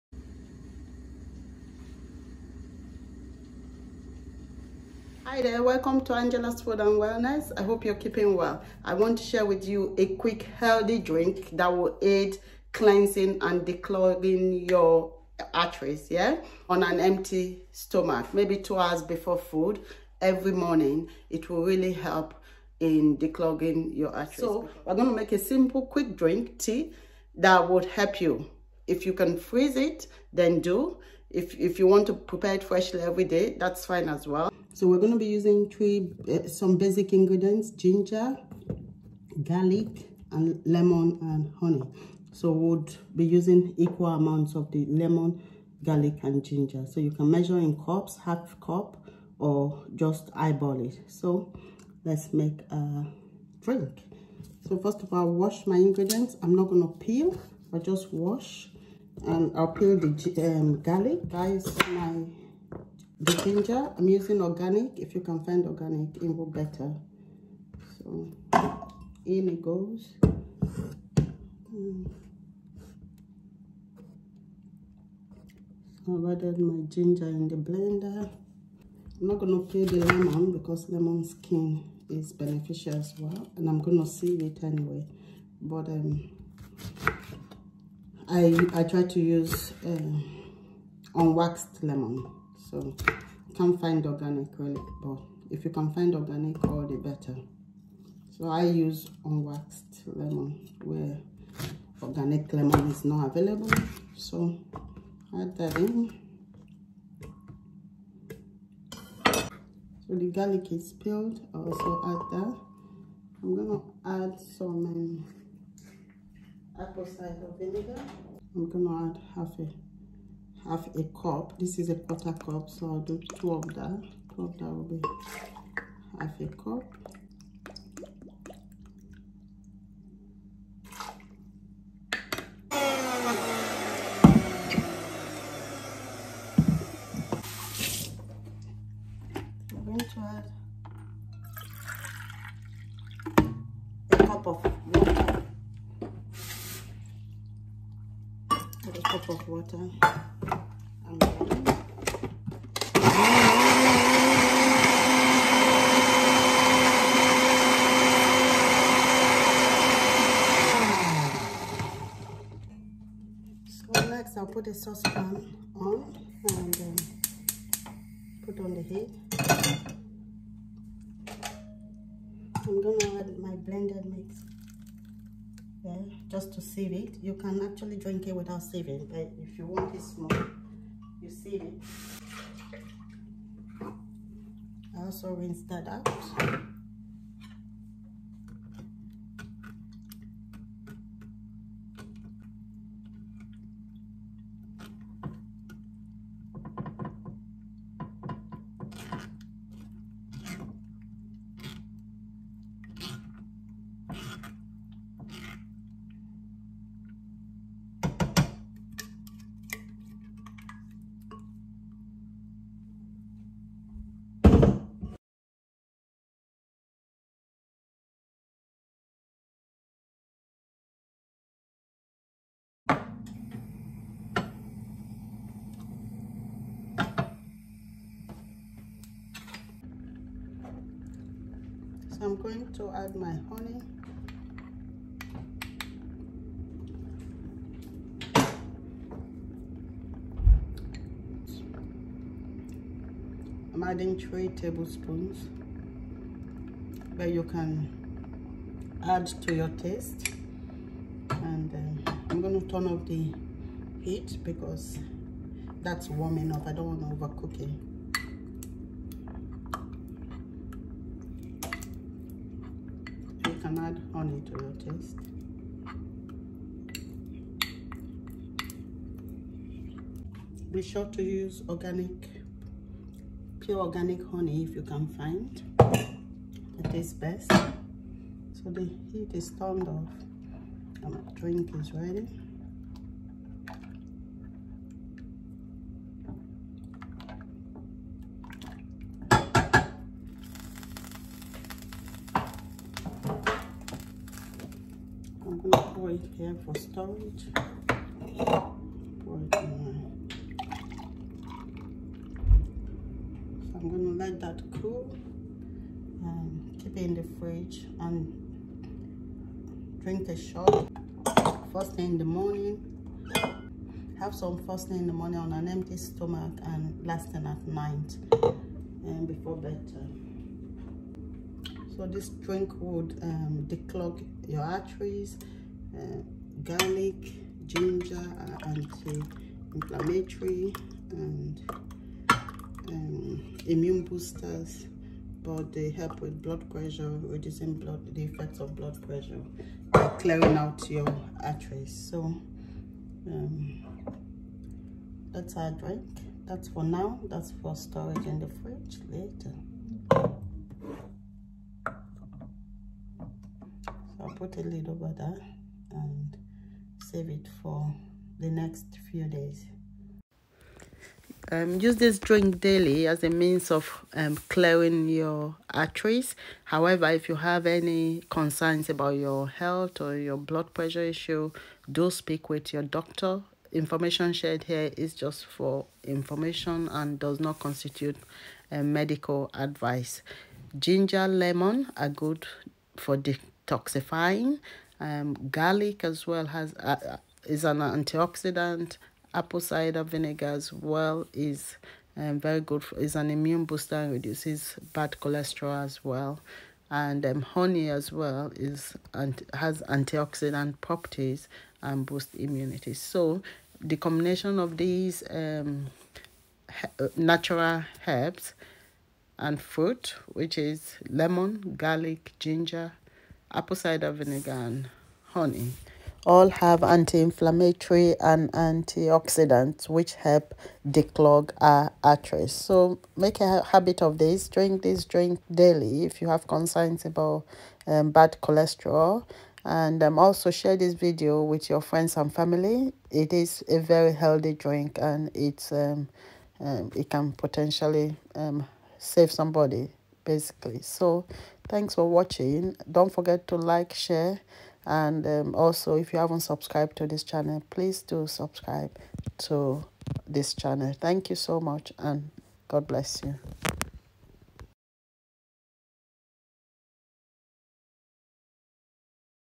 Clear Clogged Arteries with this simple drink
Ingredients:
equal amounts of:
Ginger
Garlic
Lemon
Apple Cider Vinegar
Organic Honey to taste